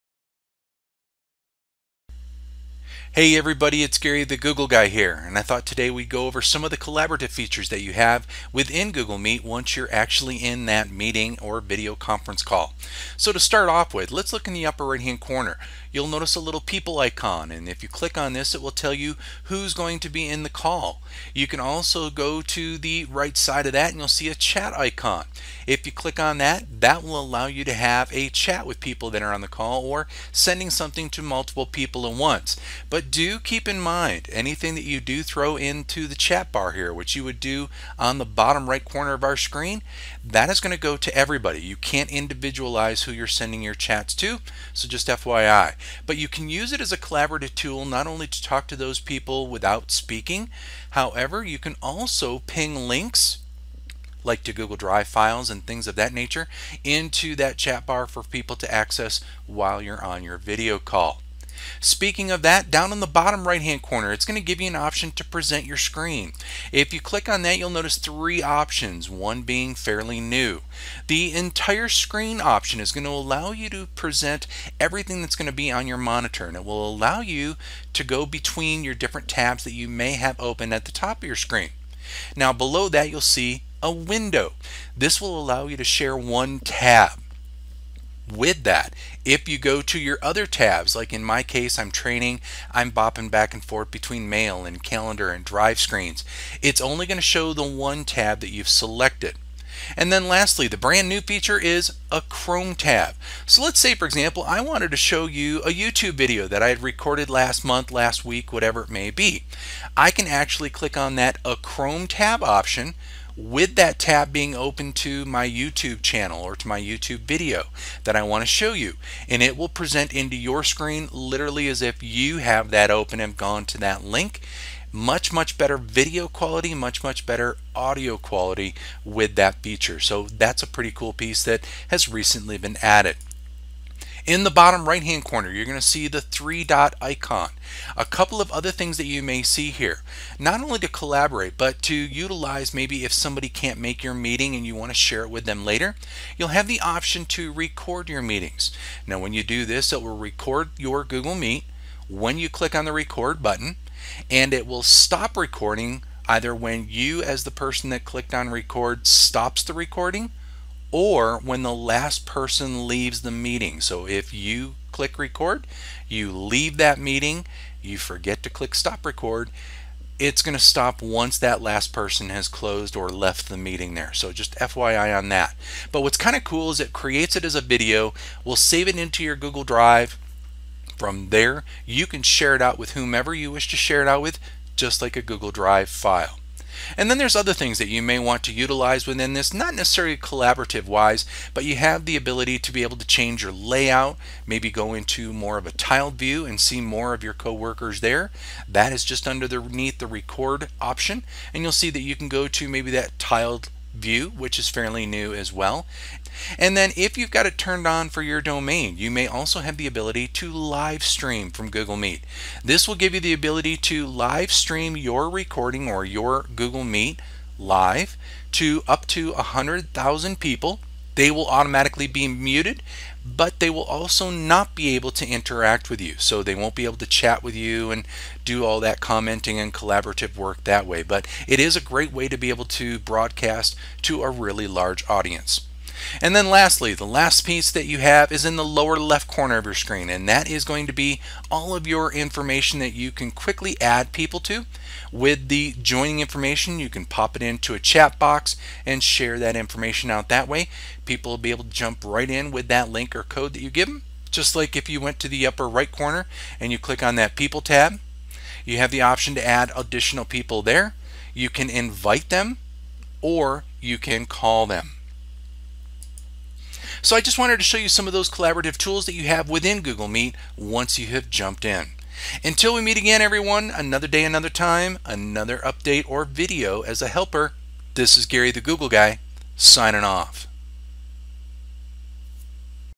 Collaborative features in Google Meet
Let's take a look at some of the collaborative features you have available to you in Google Meet. Things like adding people to meetings quickly, sharing your screen, chatting, adding links to files, recording meetings, and live stream options. If you would like a quick cheat sheet for Meet, see this document